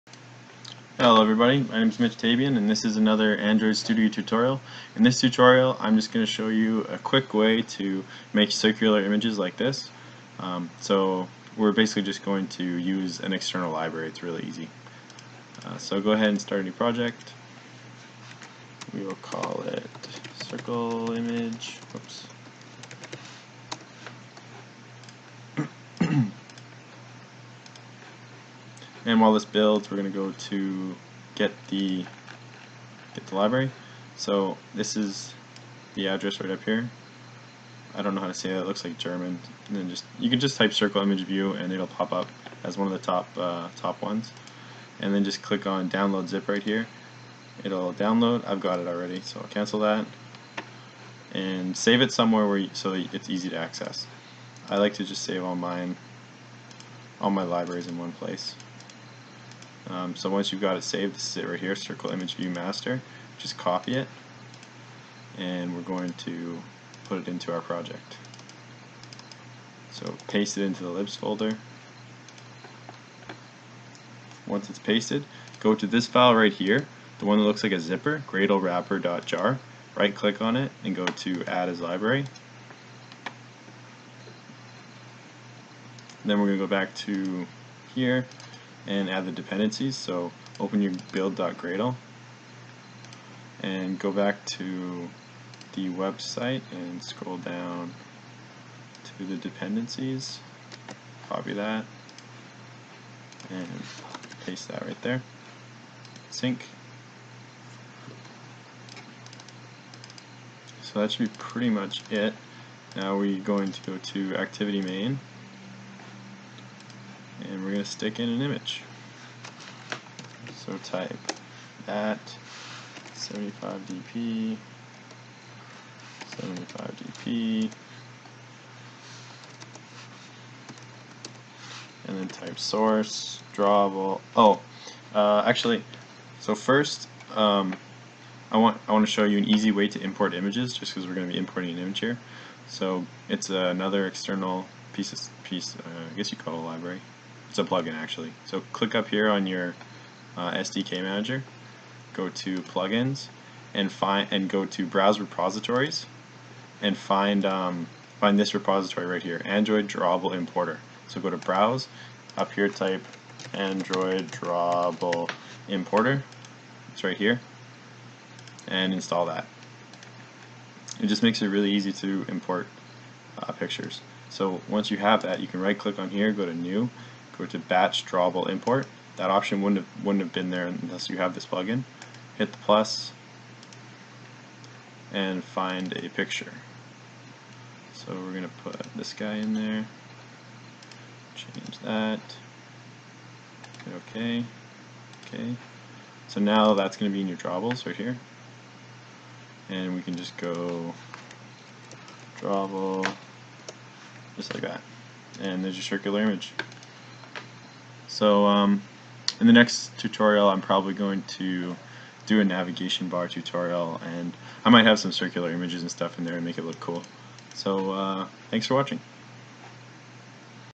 Creating Circular Images in Android Studio
Here is a quick tutorial on how to make images circular and give them a circular border in Android Studio.

Source Code: No source code, this tutorial is extremely short.